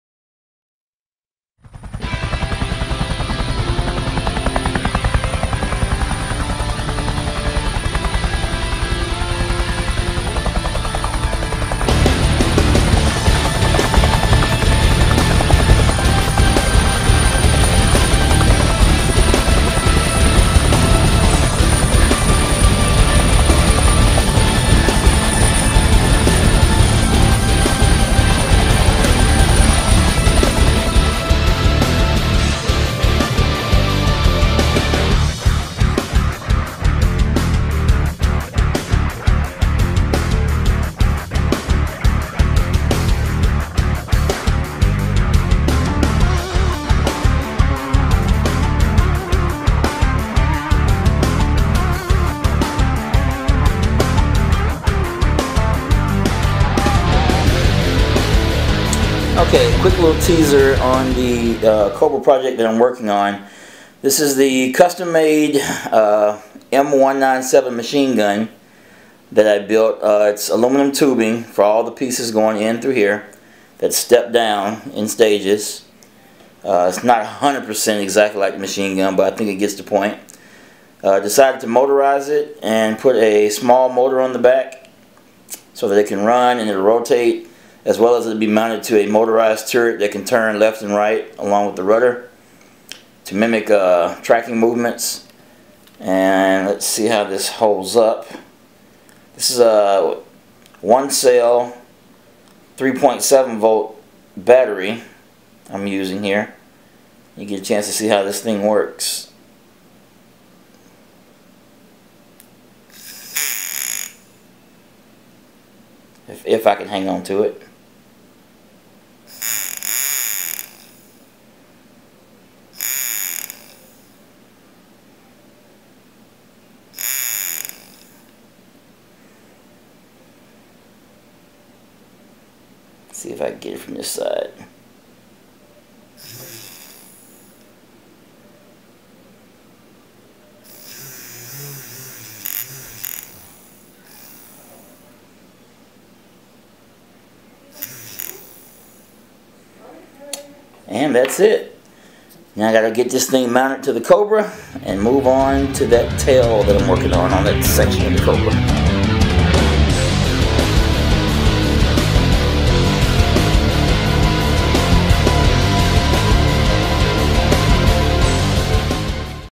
AH-1S Cobra motorized Gun Turret- Work in progress
Just a quick teaser on the build. Decided to motorize the barrels of the M197 (Custom build) AND motorize the turret itself to mirror the rudder movements. Still a lot of work to do but progress is happening. To be continued.